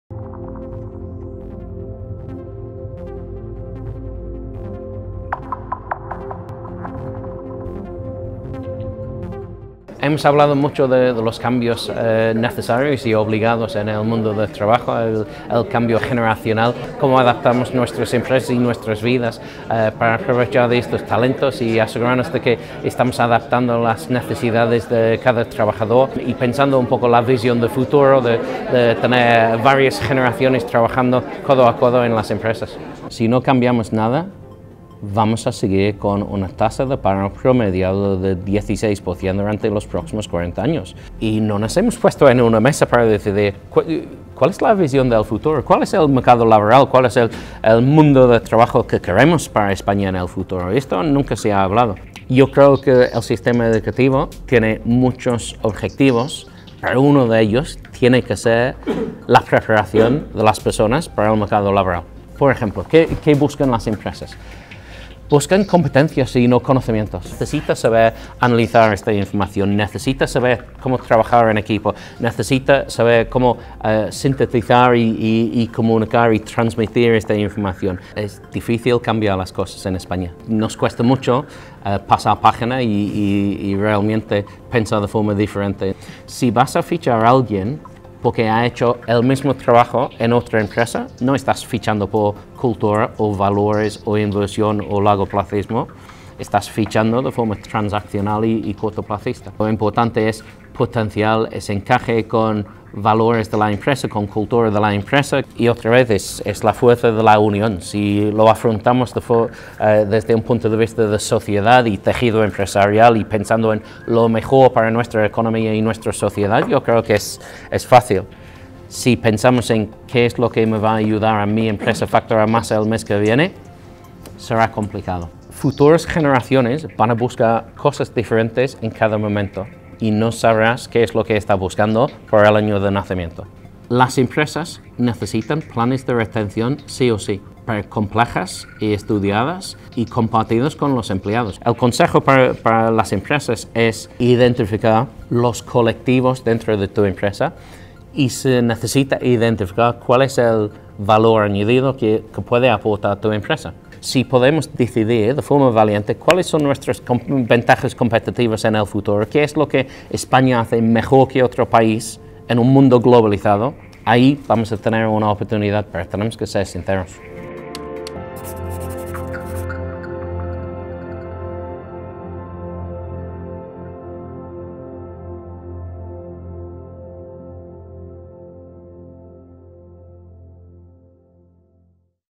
Desayuno Canal CEO con Christopher Dottie
Desayuno Canal CEO con Christopher Dottie celebrado en la Universidad Europea .